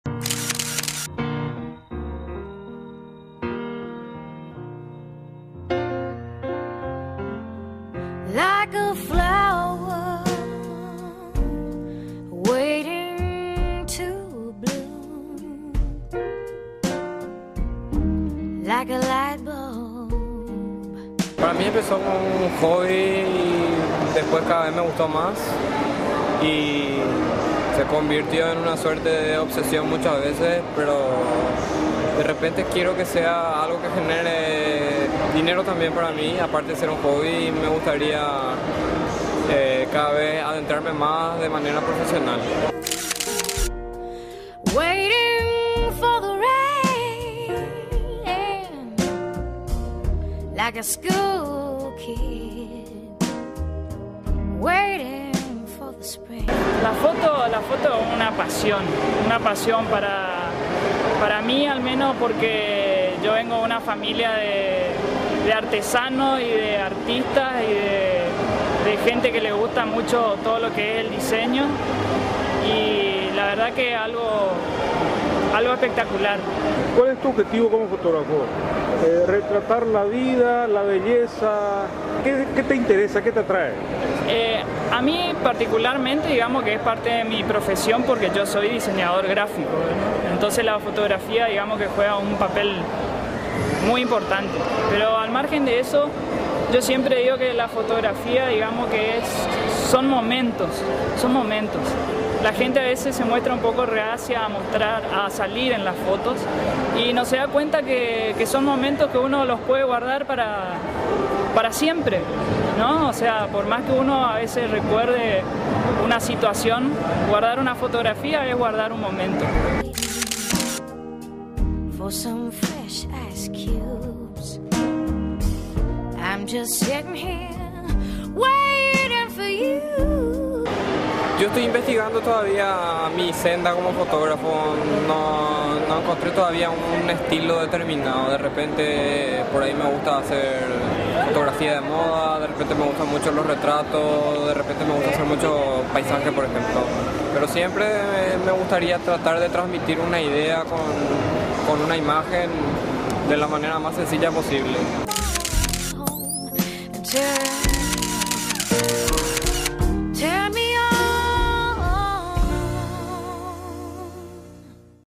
Mburucuyá: Muestra fotográfica
La primavera fue recibida de varias maneras en Asunción. Por ejemplo con una muestra fotográfica denominada Mburucuyá (nombre en guaraní de la flor pasionaria) que tuvo lugar en el Shopping del Sol desde el viernes 19 y concluye hoy domingo 21 de setiembre.